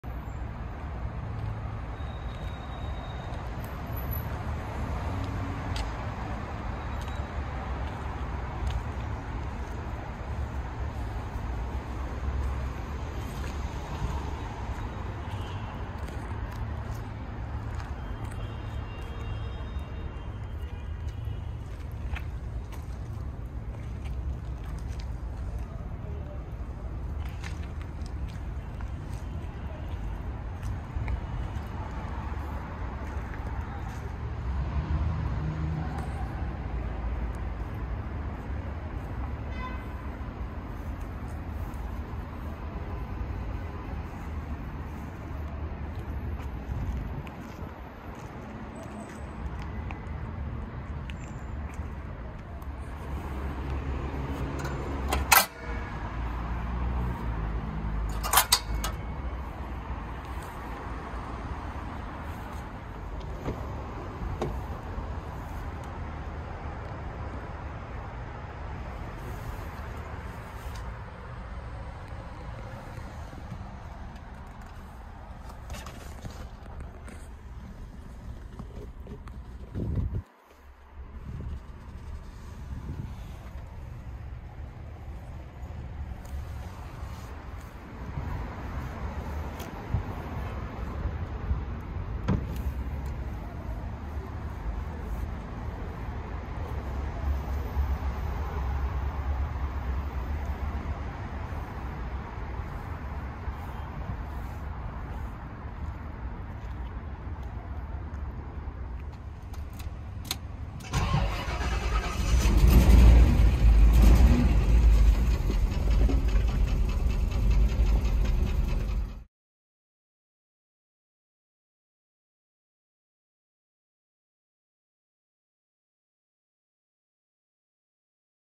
Restoration Project: 1982 Rolls Royce Silver Spur
My latest restoration project. This 82 Silver Spur was in bad condition. It was left to rot for 10+ years with a dead engine.

My first task was to revive the engine, and then continue restoring all parts.